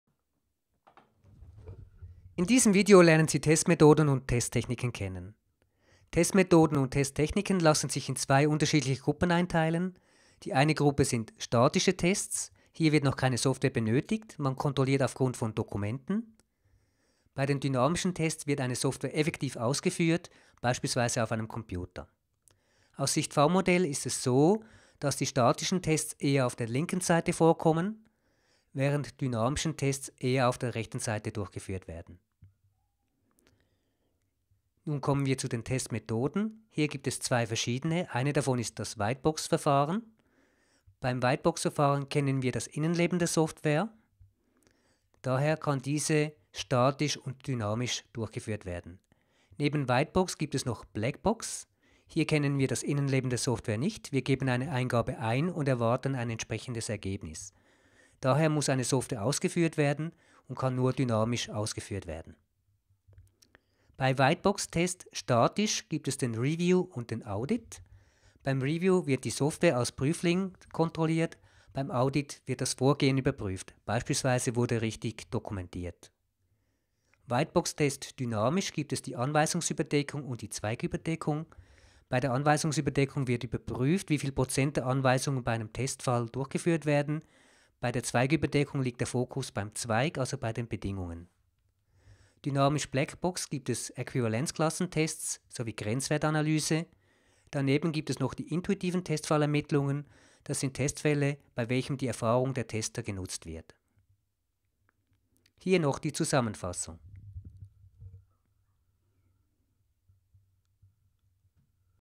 Testmethoden und Testtechniken
In diesem Video lernen Sie die unterschiedlichen Testmethoden und Testtechniken kennen. Sie erhalten einen Überblick zwischen statischen und dynamischen Tests, White Box Test sowie Black Box Test und die unterschiedlichen Testtechniken.